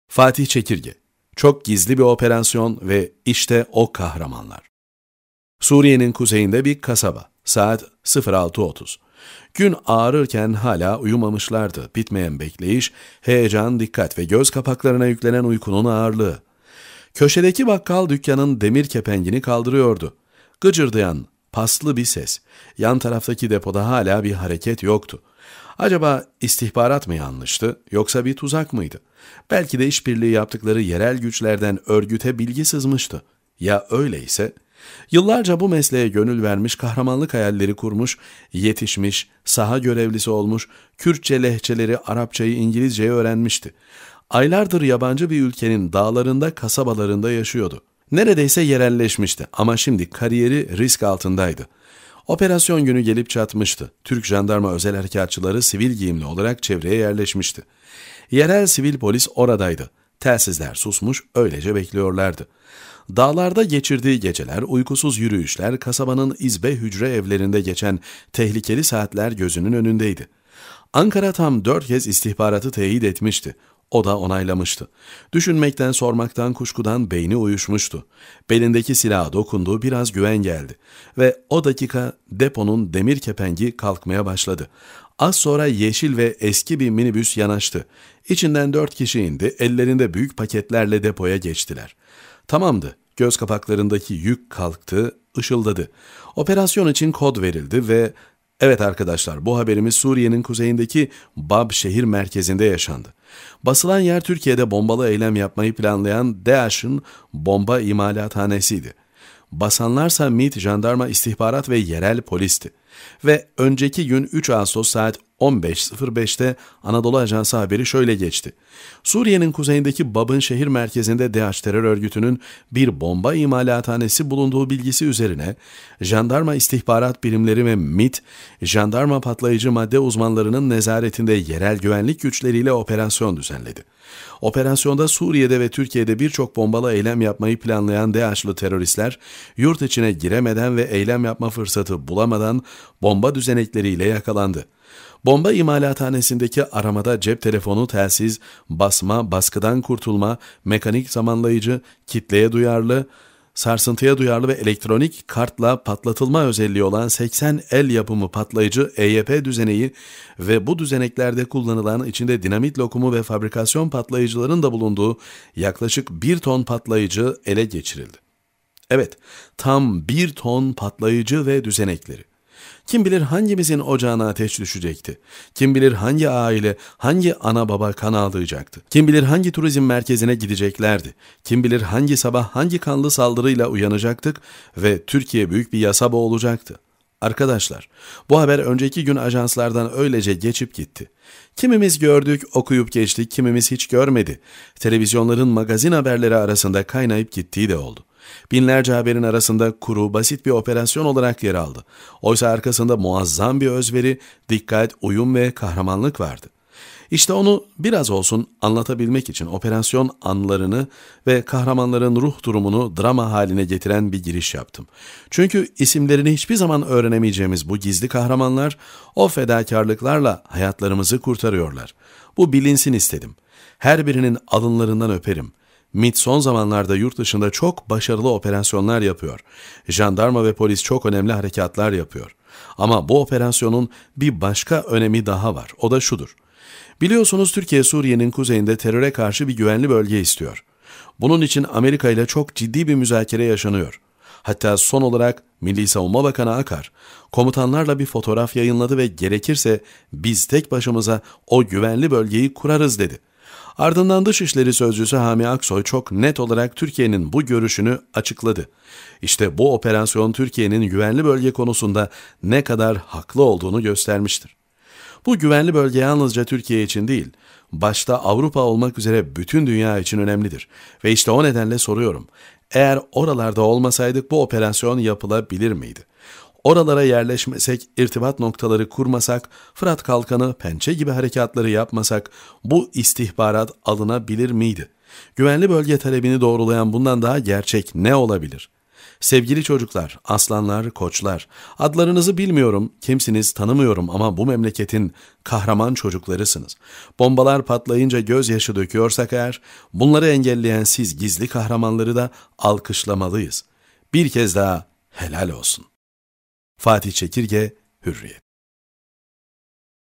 Çok gizli bir operasyon ve işte o kahramanlar... Sesli Makale
"Aylardır yabancı bir ülkenin dağlarında, kasabalarında yaşıyordu...
Neredeyse yerelleşmişti.
Ama şimdi kariyeri risk altındaydı...
Operasyon günü gelip çatmıştı. "